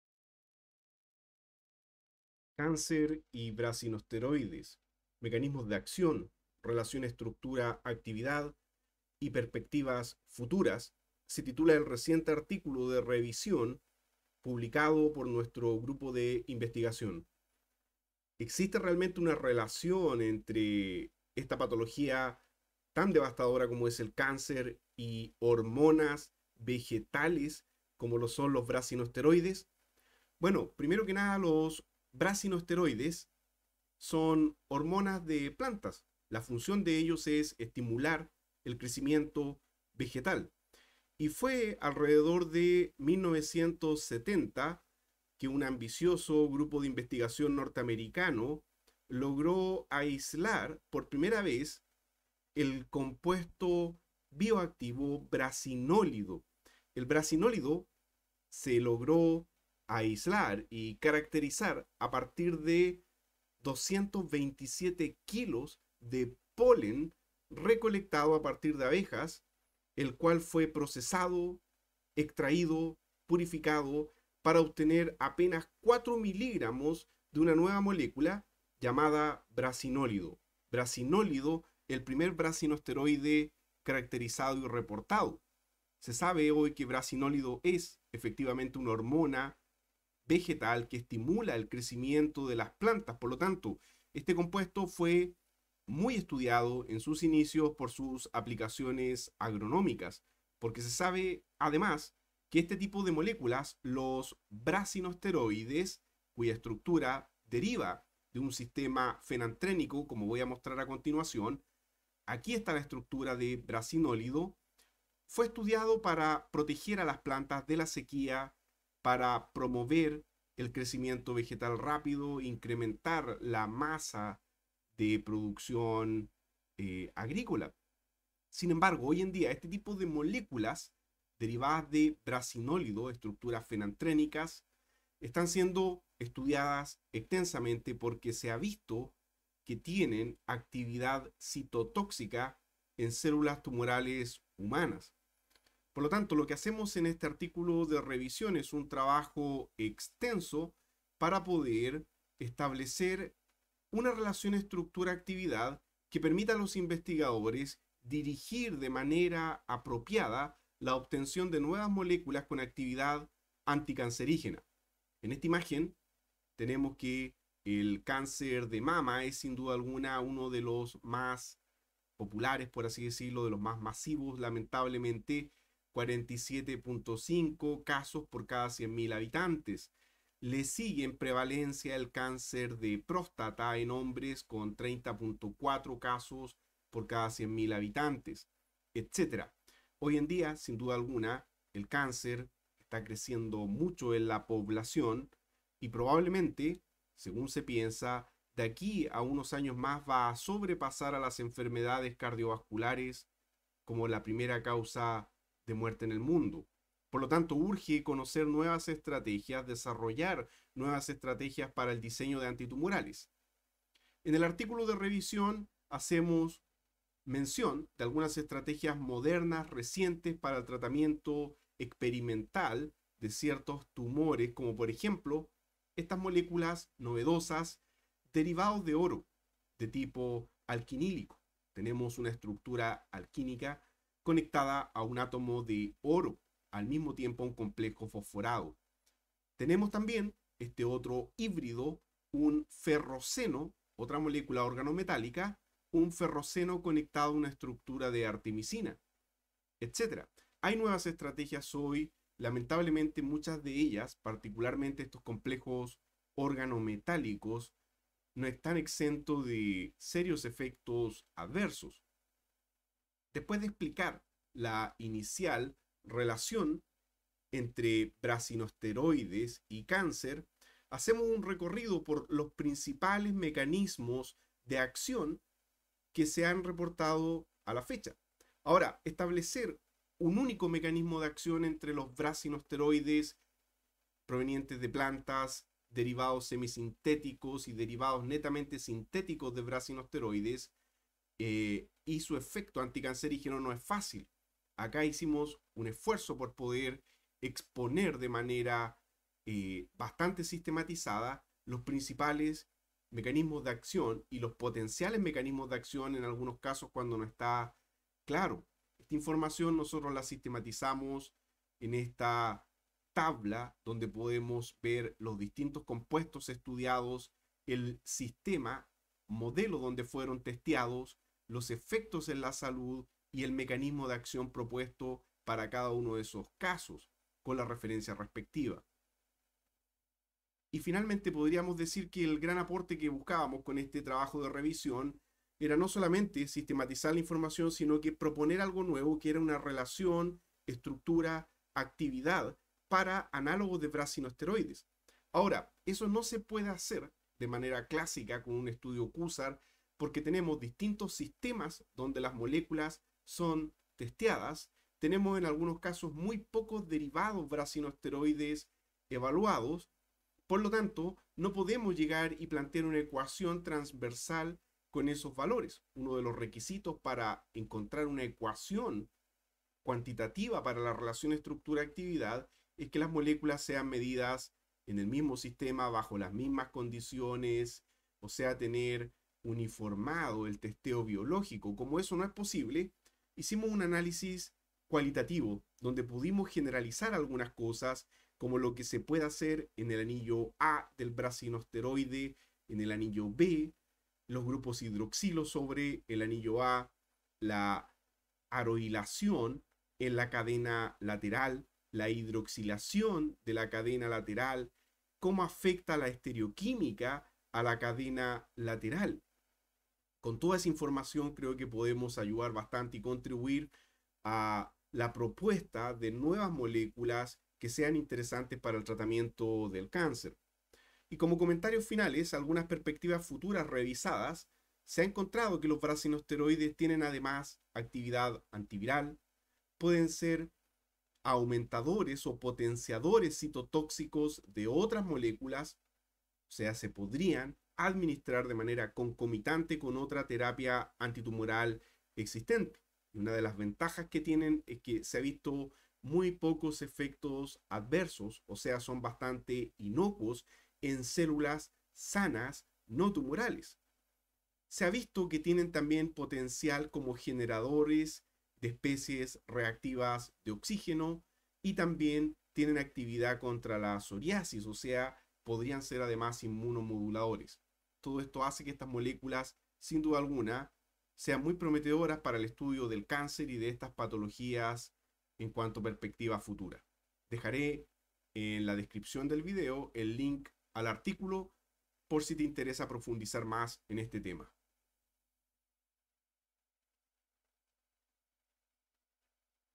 Hormonas de plantas para el tratamiento del Cáncer - Cancer and Brassinosteroids Plant Hormones
Cáncer y Brasinoesteroides, mecanismos de acción, relación estructura-actividad y perspectivas futuras.
Cancer and brassinosteroids: Mechanisms of action, SAR and
future perspectives: